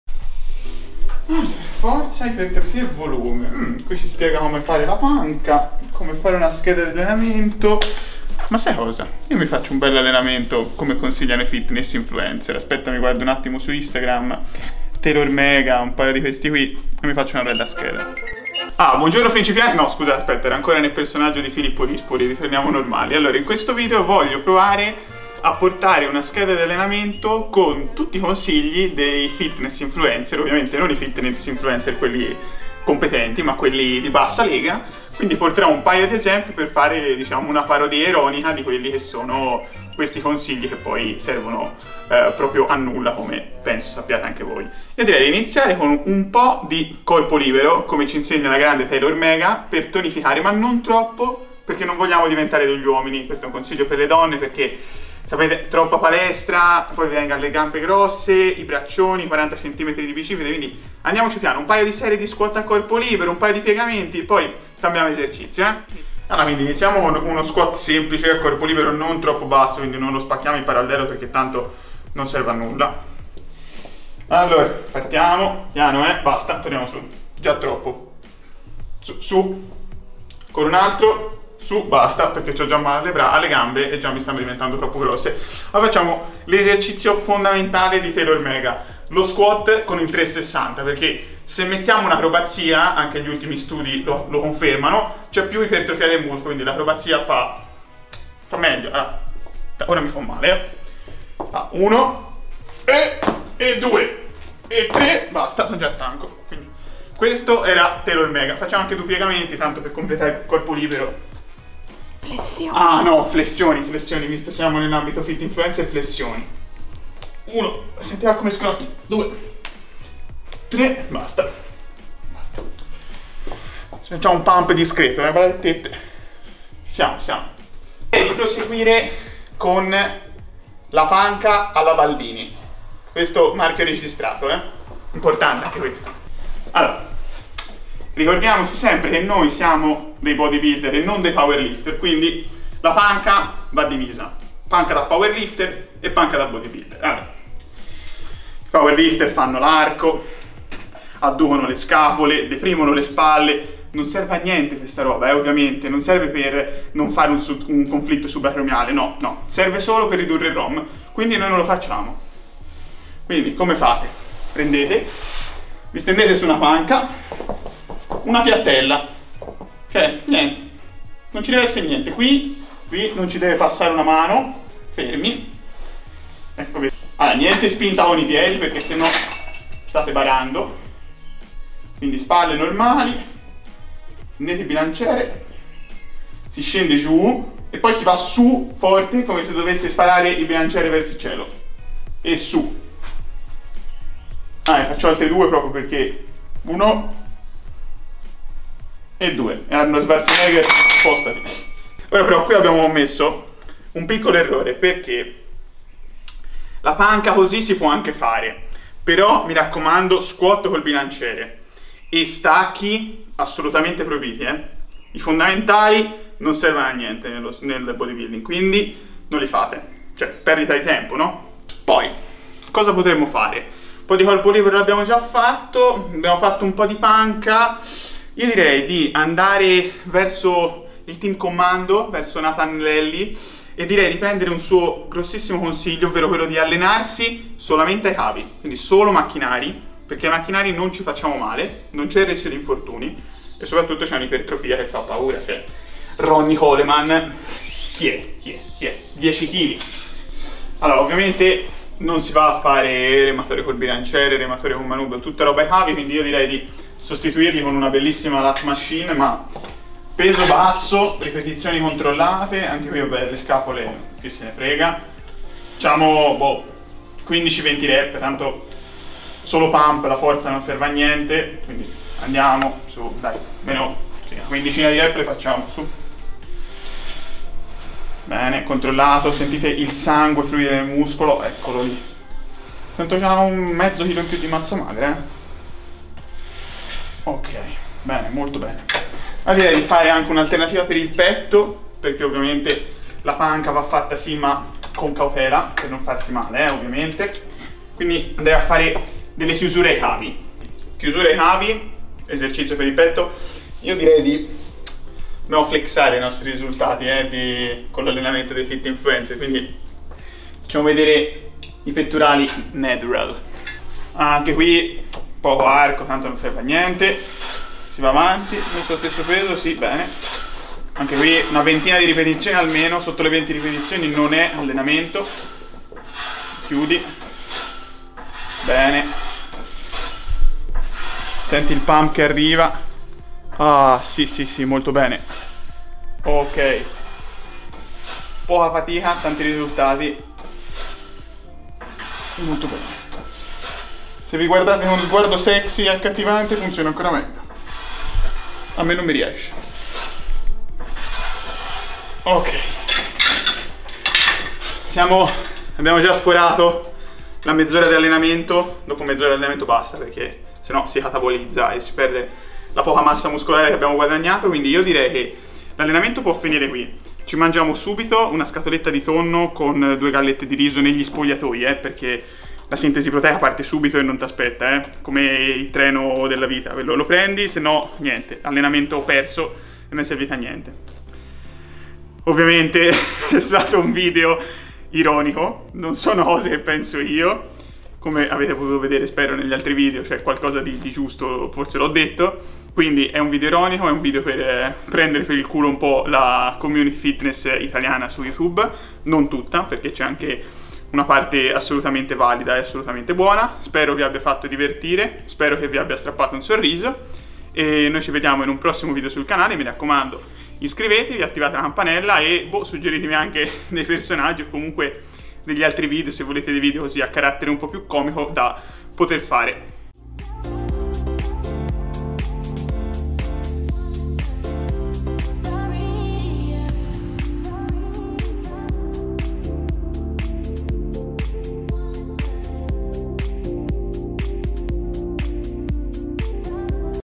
MI ALLENO CON I CONSIGLI DEI FIT INFLUENCER (Nathan Warmachine,Rispoli,Taylor Mega,Baldini)🏋️‍♀️
oggi un video diverso dal solito, un video a sfondo comico per mettere in luce le fake news dei fit influencer a tema allenamento, infatti andrò a replicare un allenamento con i consigli dei fit influencer più in voga del momento ovverò Baldini. Nathan, Taylor e Rispoli, come sarà andata?
sarò diventato secco?
spero che questo contenuto vi strappi un sorriso in un momento così difficile, un modo alternativo di fare dissing.